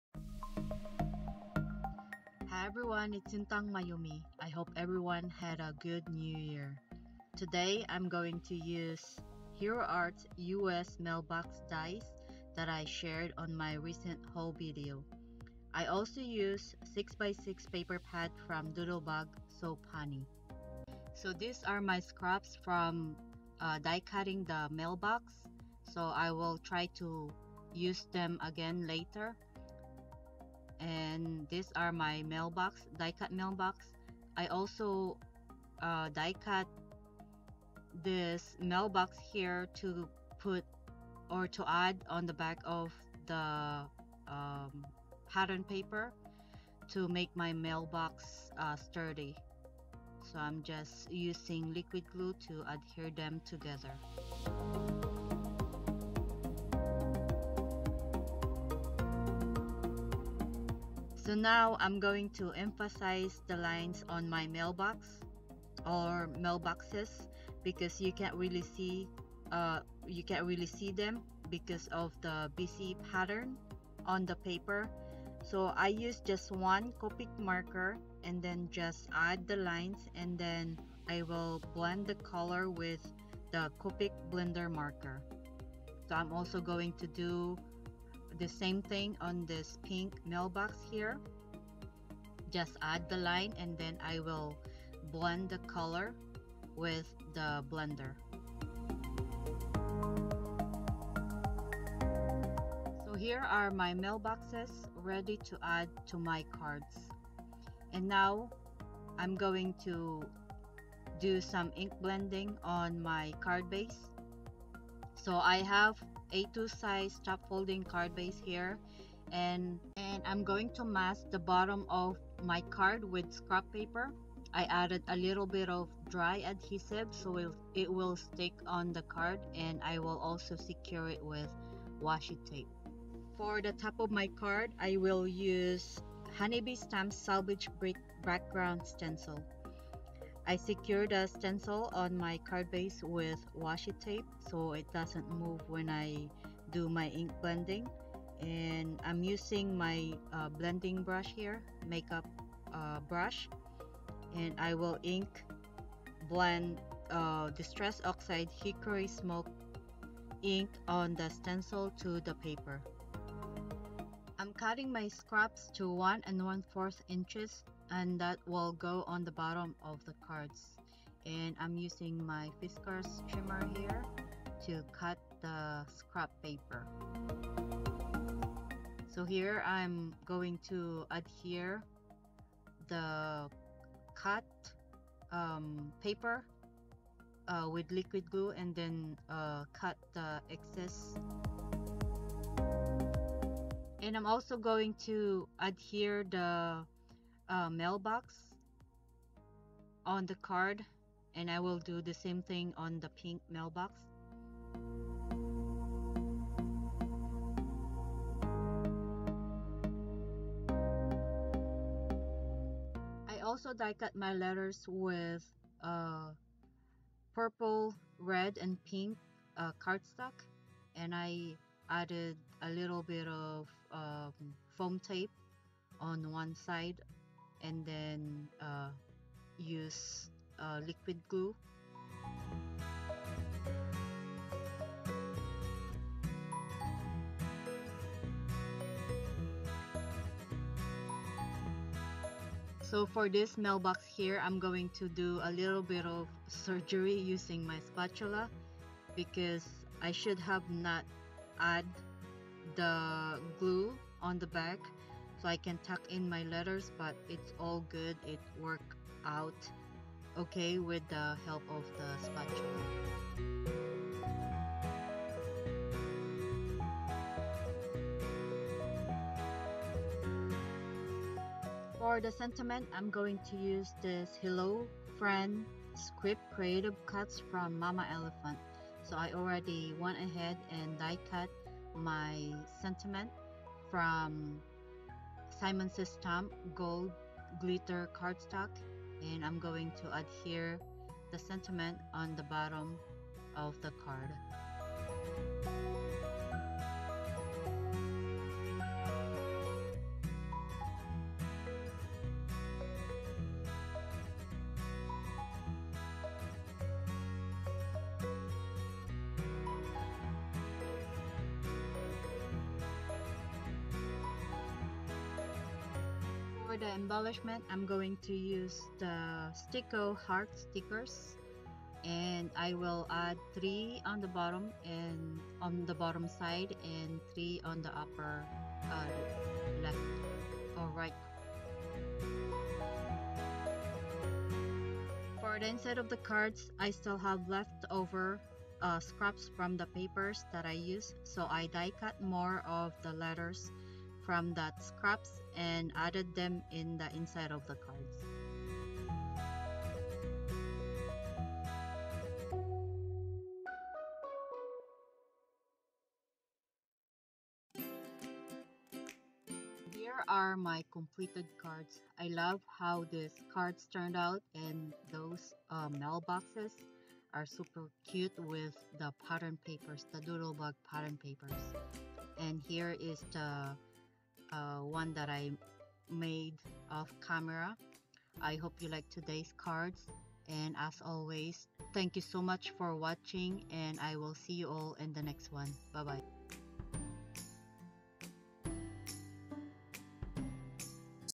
Hello Friend Cards Using Hero Arts US Mailbox Die And Doodlebug So Punny Papers
Hi friends! I hope everyone had a good New Year. I have my very first video for 2021. I hope you like these cards as much as I do.

Products Used:

Hero Arts US Mailbox Fancy Die

Art Glitter Glue Designer Dries Clear Adhesive

Blending Brush

TH Hickory Smoke Oxide Ink

Fiskars Bypass 9 in. Trimmer

Sticko Heart Stickers

Products I Used For Filming:

--GoPro Hero 5--
--Desk Clamp To Hold The Camera--
--Memory Card--
--Multi Card Reader--
--Canon EOS Rebel T5--